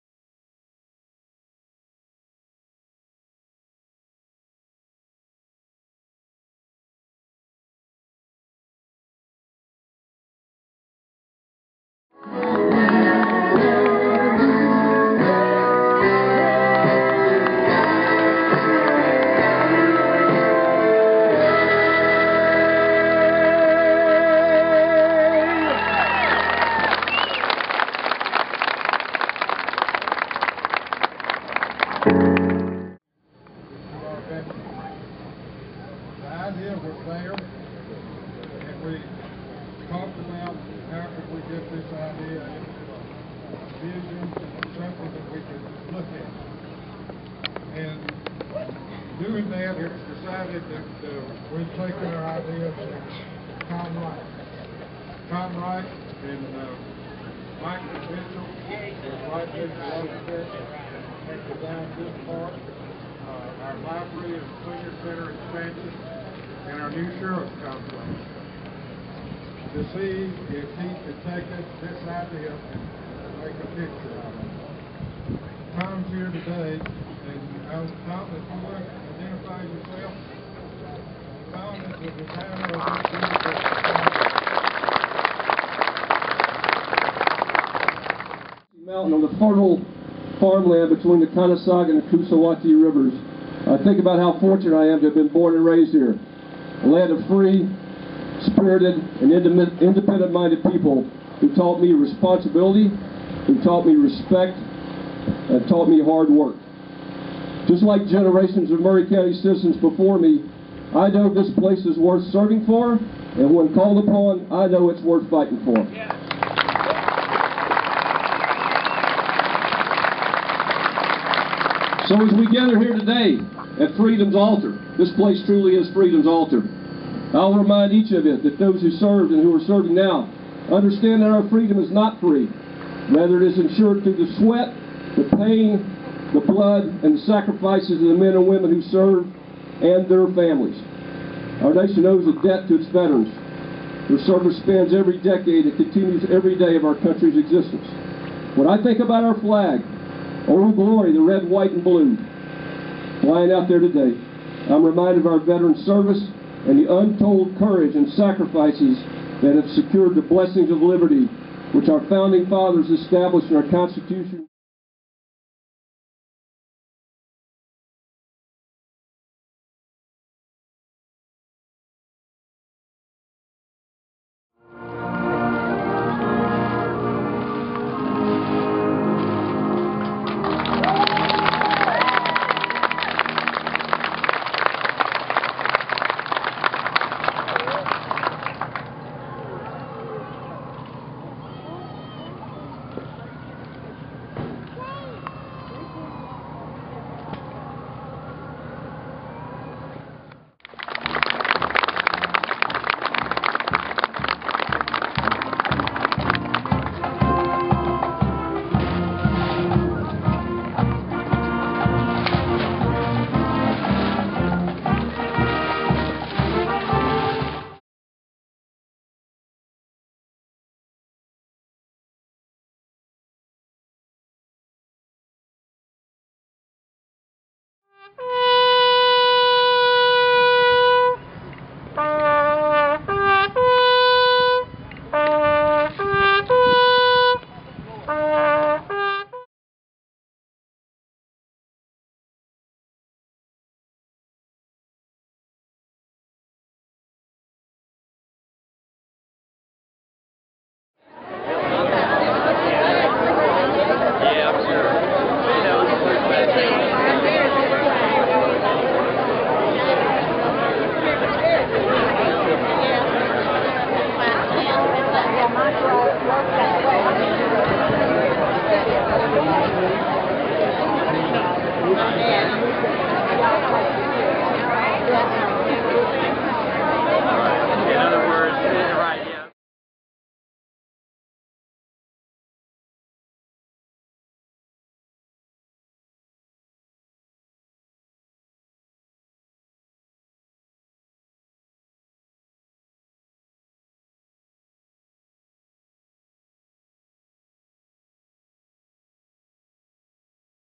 Murray County Veterans Memorial Park
The Murray County Veterans Memorial Park is dedicated on Saturday, May 29, 2010.